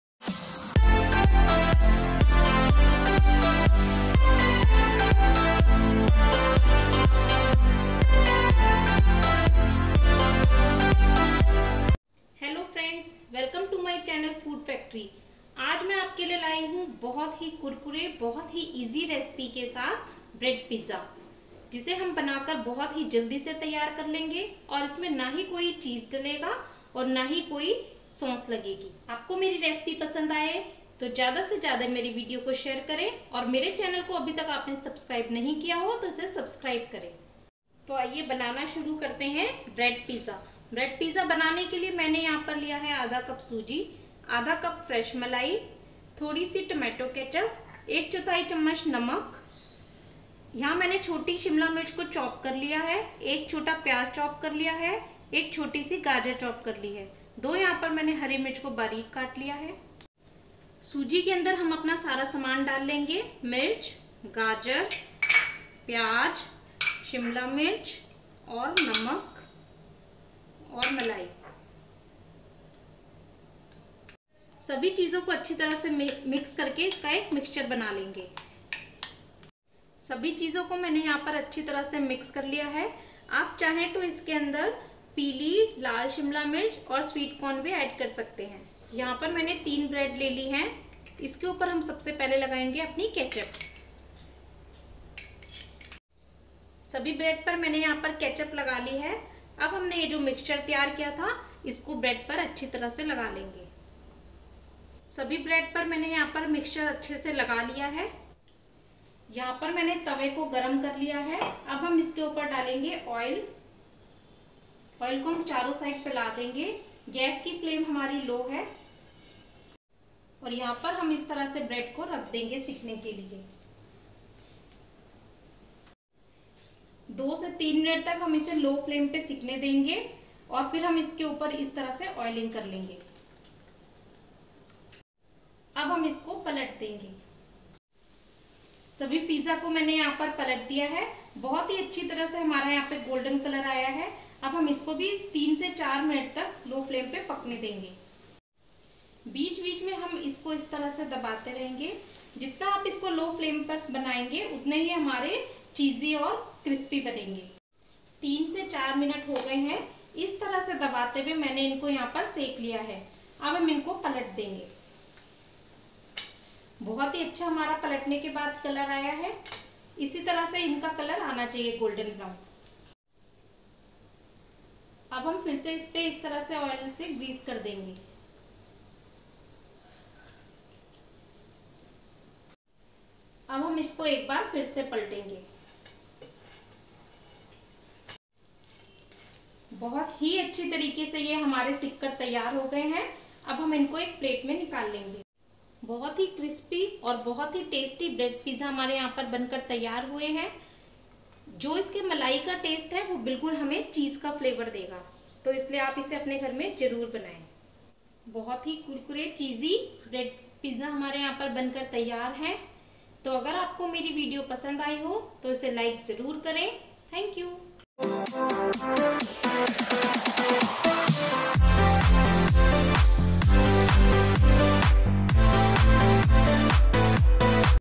इस तरह से घर पर बनाए सूजी का ब्रेड पिज़्ज़ा
Ingredients required for making the bread pizza are...

1/2 Cup Suji
1/2 Cup Malai
Tomato Ketchup
1/4 tsp Salt
Chopped Capsicum
Chopped Onion
Chopped Carrot
2 Chopped Green chilli